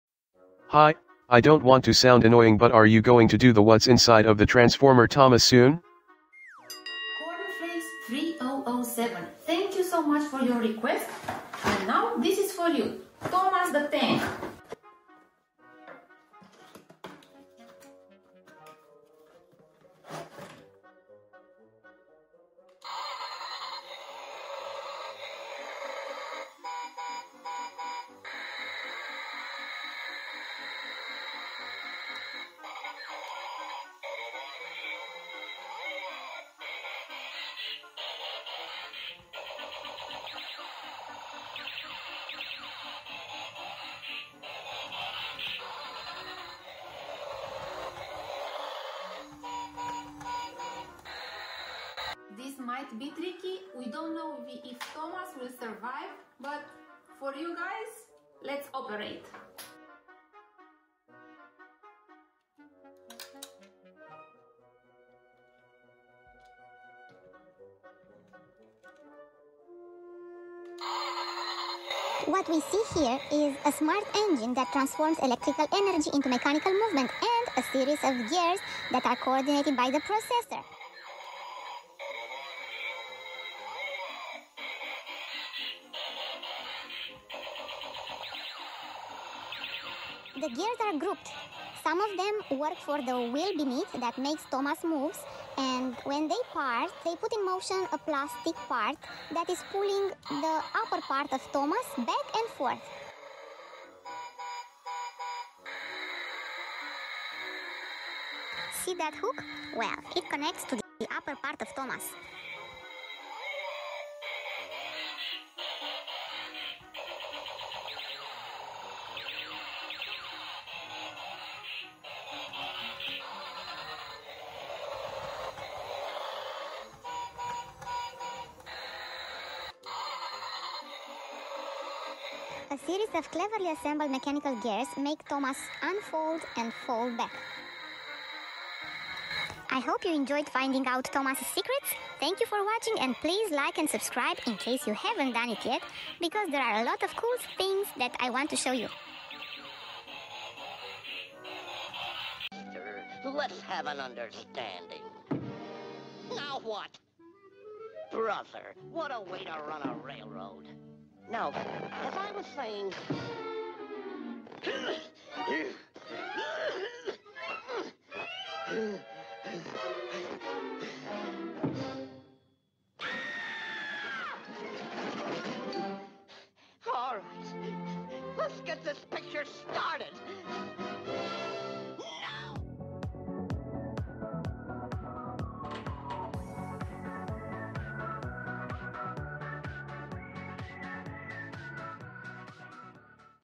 What's inside THOMAS the Tank
I was pretty scared I would not be able to put it back together, but it turned out to be a new interesting experience for me too! Check out the clip and find out the principle of how transformers toy with batteries operate!